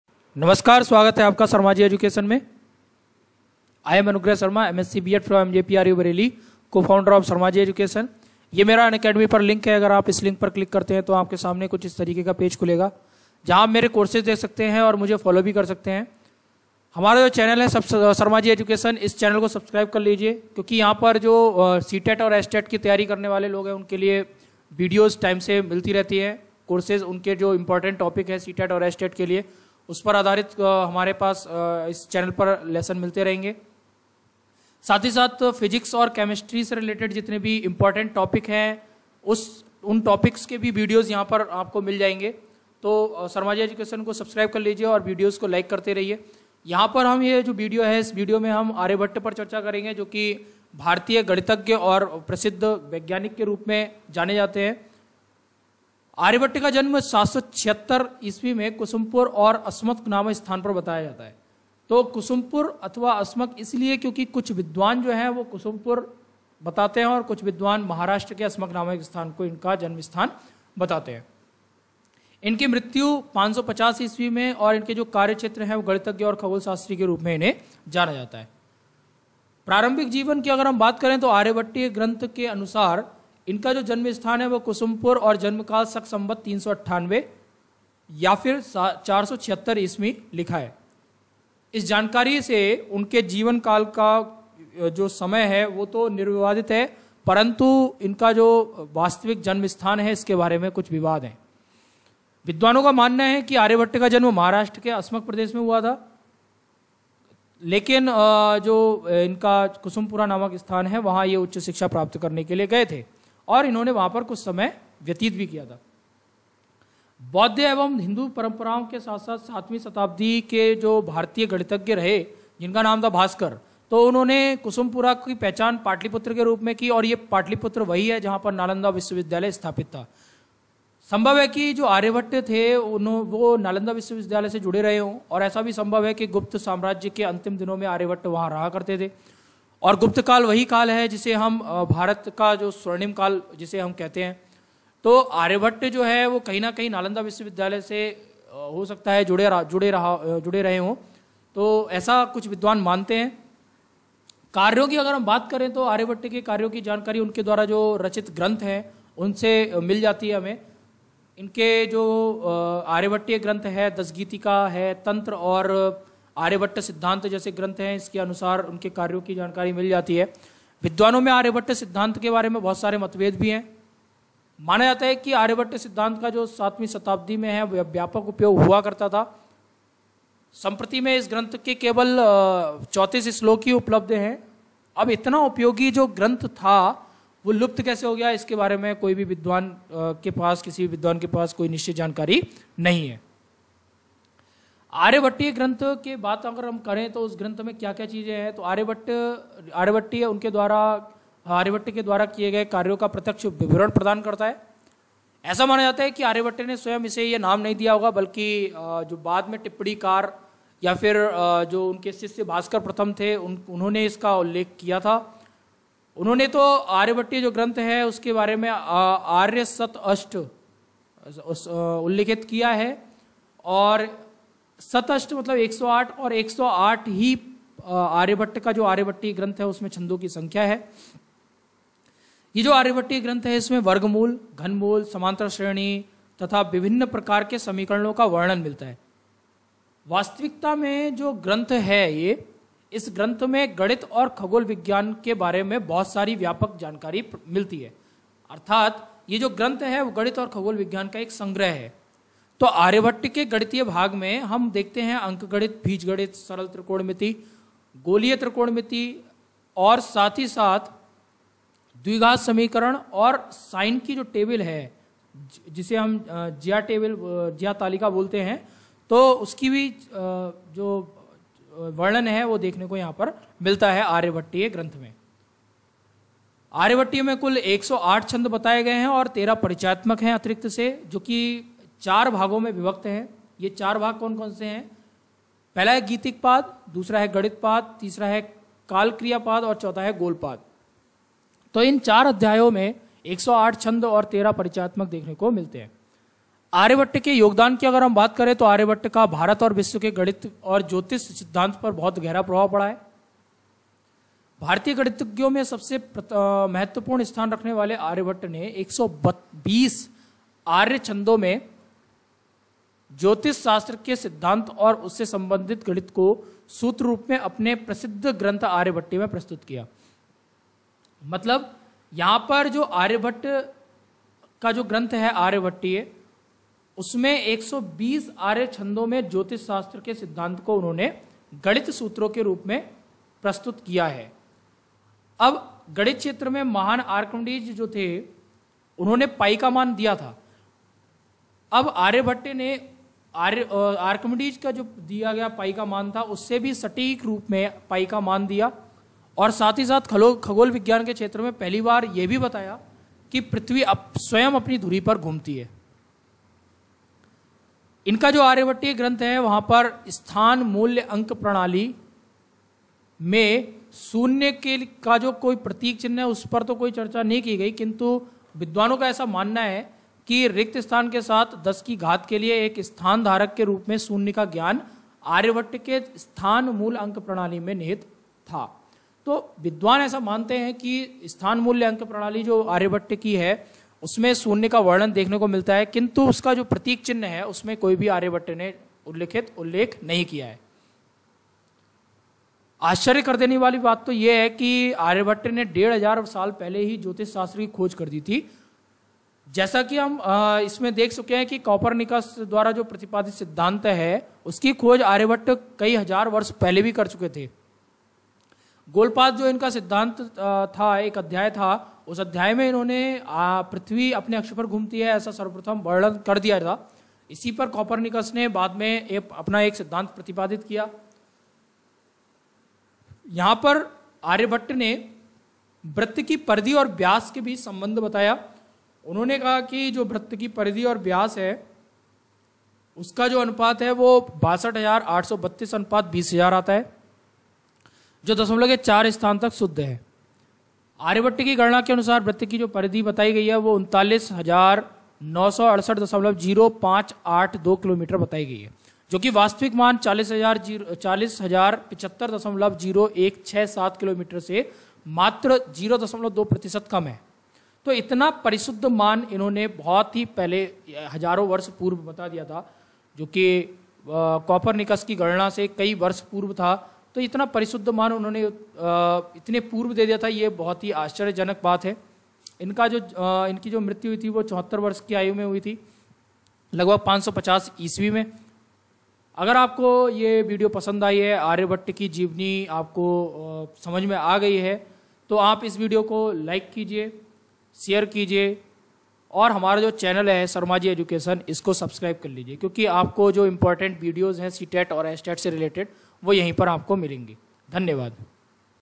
आर्यभट्ट: एक गणितज्ञ तथा वैज्ञानिक CTET, STET, SSC, RAILWAY etc
In this video we will discuss about Aarybhatt.